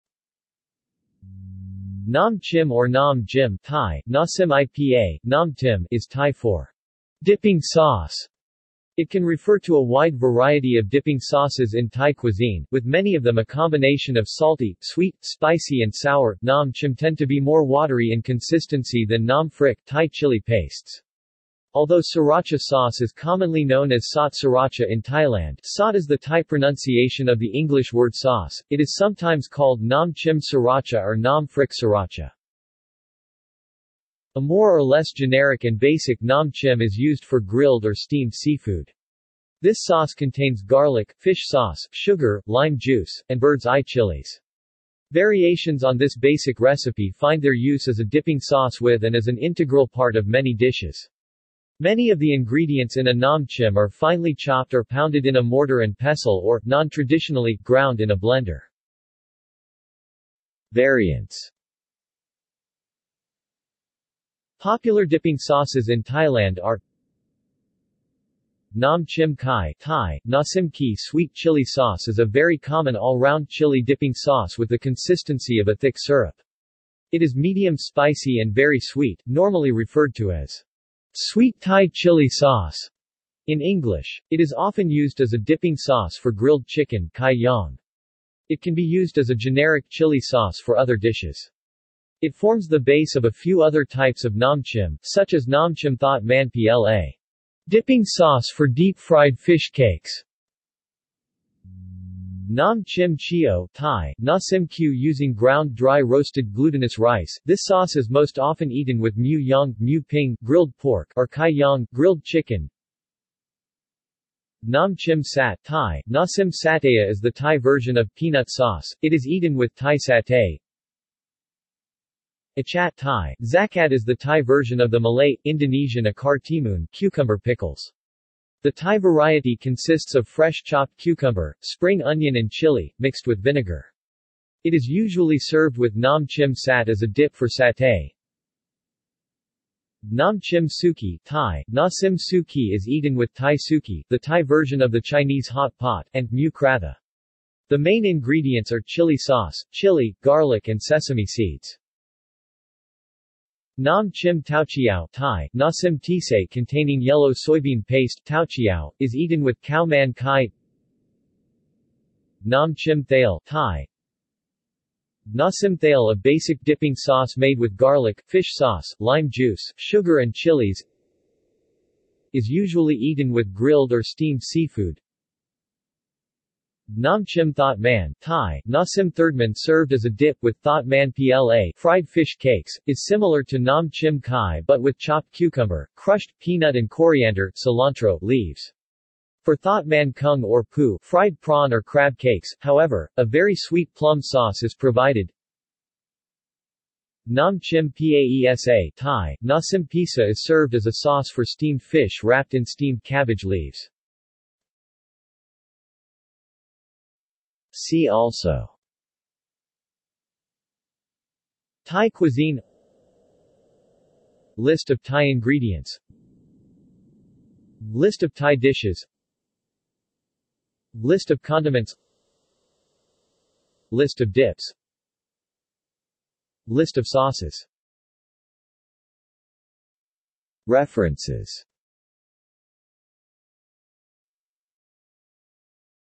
Nam chim | Wikipedia audio article
This is an audio version of the Wikipedia Article:
Nam chim

SUMMARY
Nam chim or nam jim (Thai: น้ำจิ้ม, IPA: [nám tɕîm]) is Thai for "dipping sauce". It can refer to a wide variety of dipping sauces in Thai cuisine, with many of them a combination of salty, sweet, spicy and sour.Nam chim tend to be more watery in consistency than nam phrik (Thai chili pastes). Although Sriracha sauce is commonly known as sot Sriracha in Thailand (sot is the Thai pronunciation of the English word sauce), it is sometimes called nam chim Sriracha or nam phrik Sriracha.
A more-or-less generic and basic nam chim is used for grilled or steamed seafood. This sauce contains garlic, fish sauce, sugar, lime juice, and bird's eye chilies. Variations on this basic recipe find their use as a dipping sauce with and as an integral part of many dishes. Many of the ingredients in a nam chim are finely chopped or pounded in a mortar and pestle or, non-traditionally, ground in a blender.